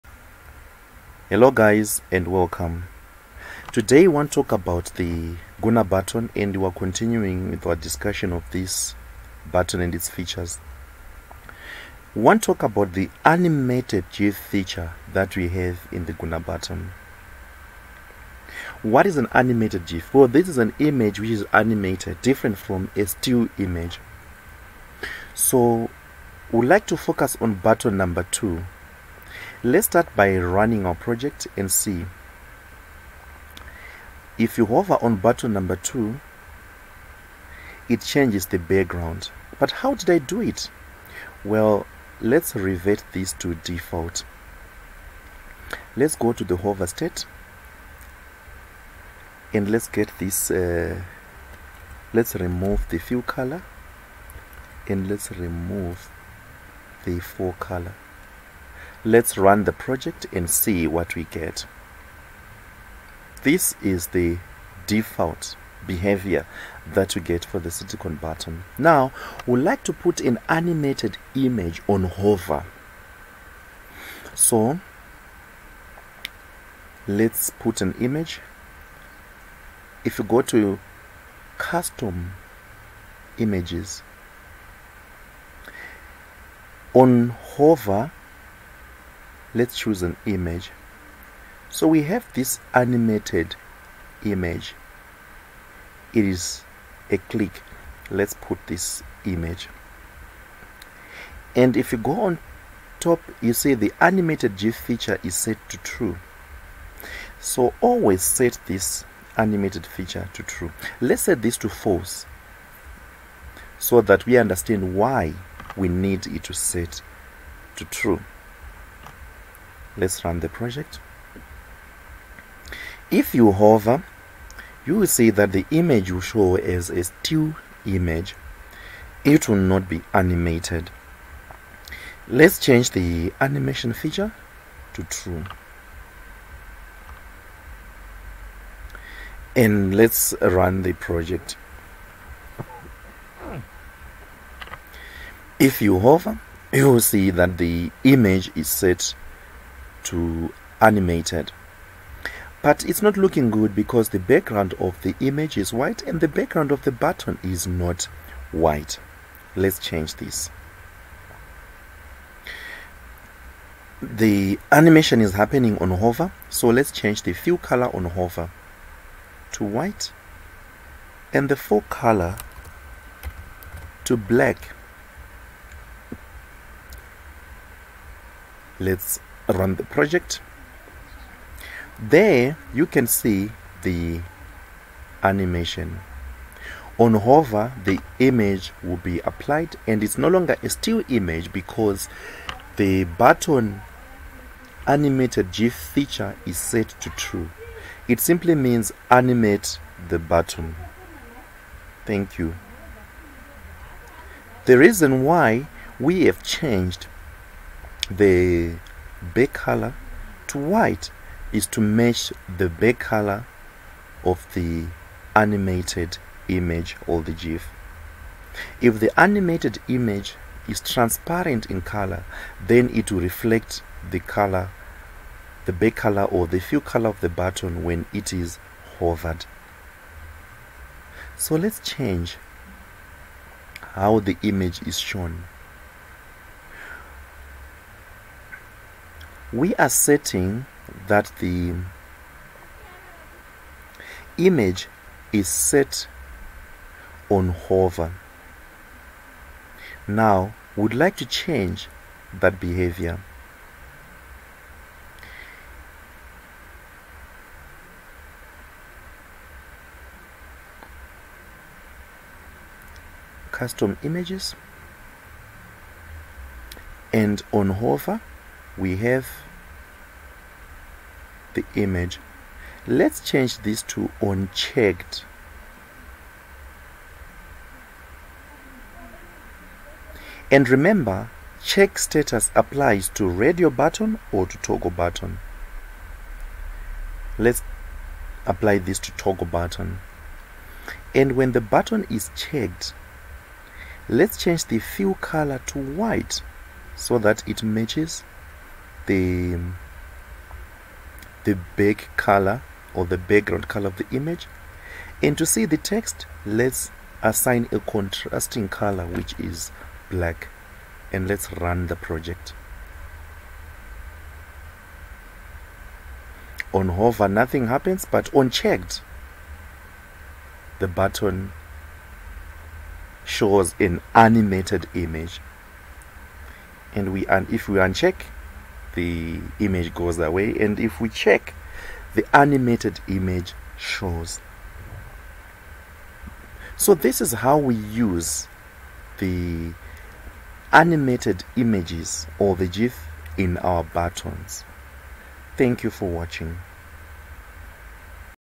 Guna UI2 Button Control
Learn how to use the button control:
# Custom Images
# Animated GIF